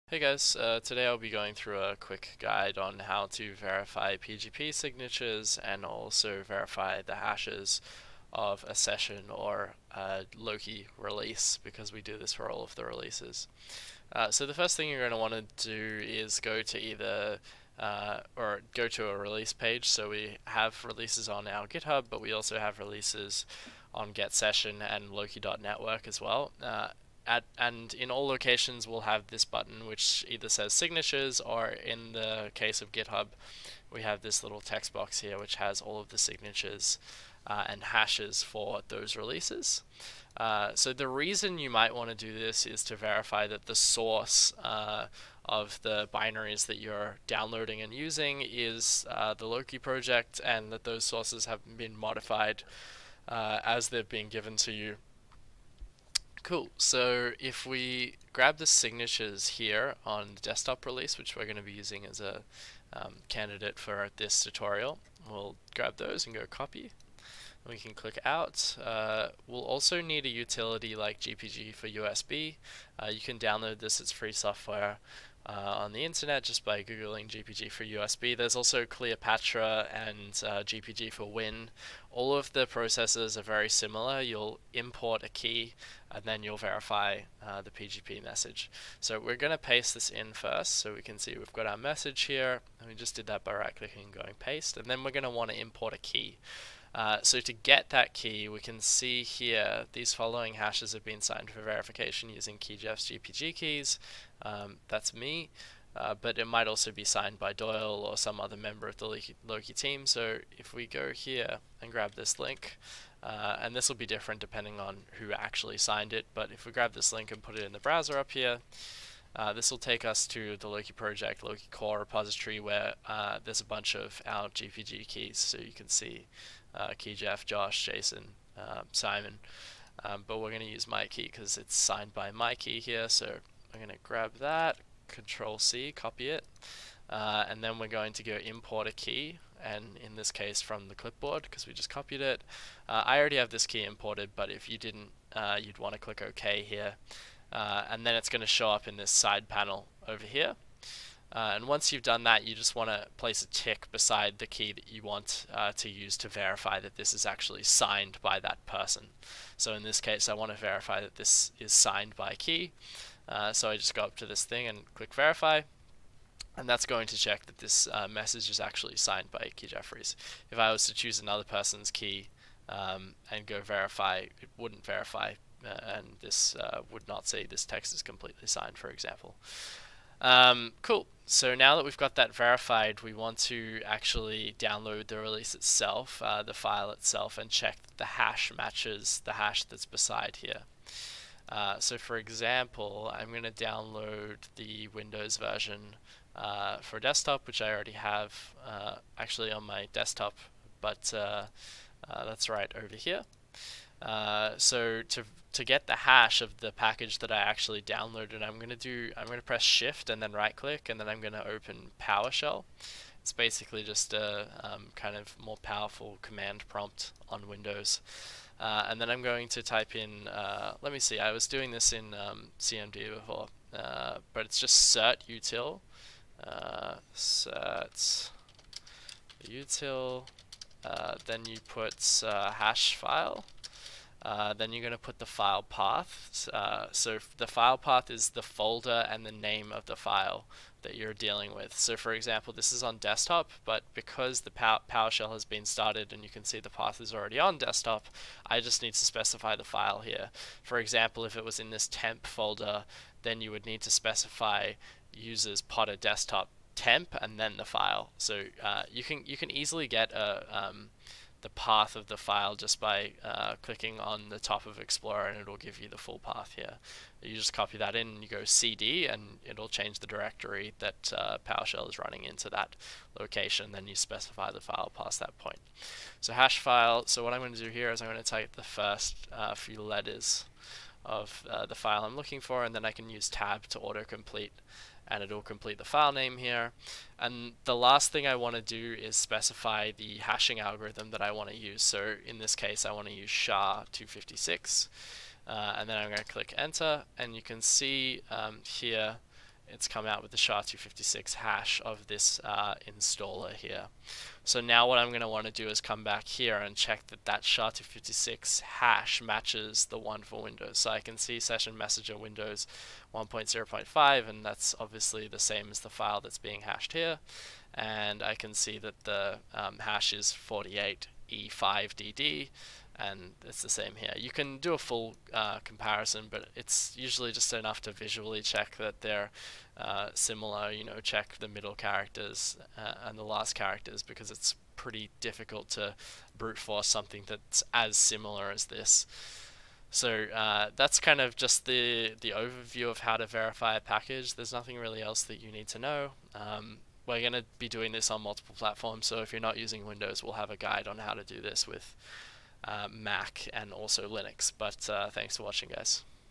Session | Verifying Session hashes on Windows
Guide for verifying Session hashes on Windows using GPG.

All of our Session releases are accompanied by a set of hashes, so that you can verify the releases as legitimate Loki Foundation versions.

This guide is for Windows, but we also have guides for Linux and MacOS. These same steps can be used to with other software to verify that it is coming from a trusted source.